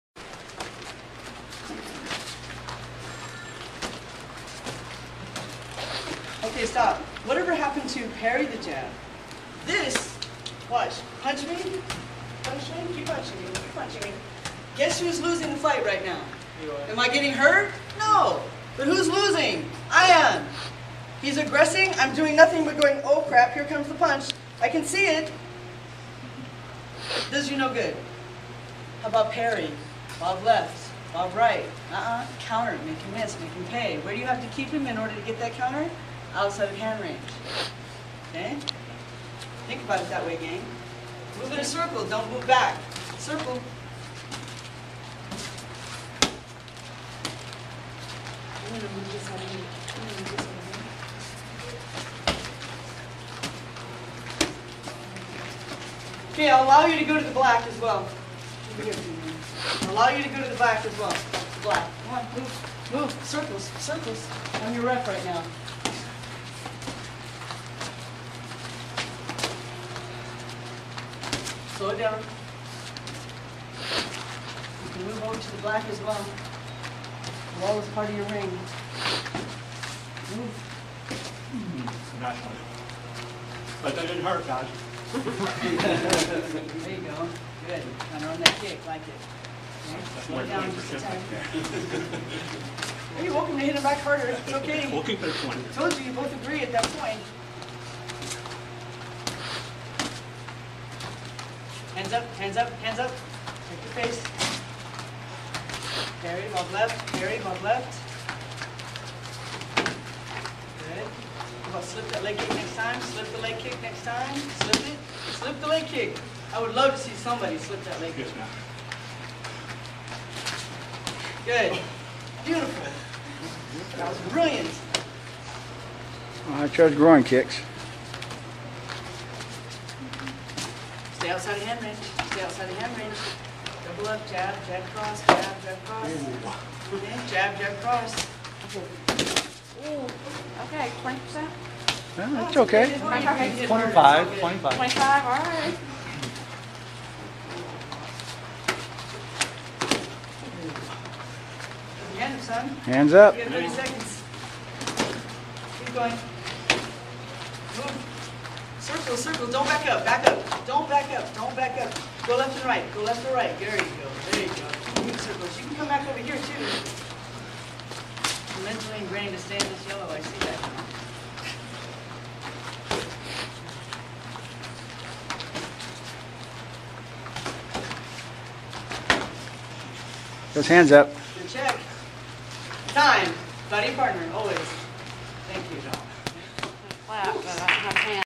KathyLong SeminarSparring 1
TKD students trying to use some of the techniques Kathy Long teaches at her seminar in Houston, TX.  Everyone had a blast.